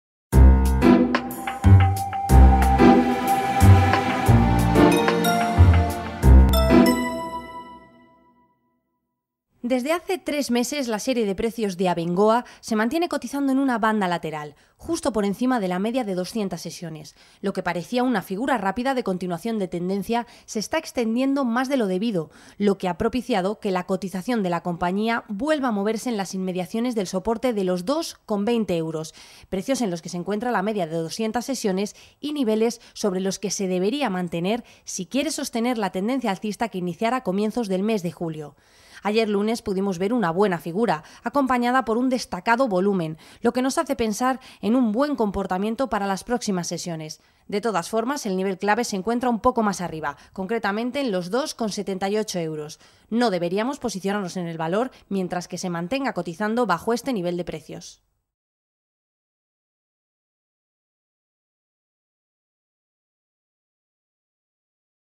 Análisis técnico de Abengoa
Ayer lunes pudimos ver una buena figura acompañada por un destacado volumen lo que nos hace pensar en un buen comportamiento para las próximas sesiones.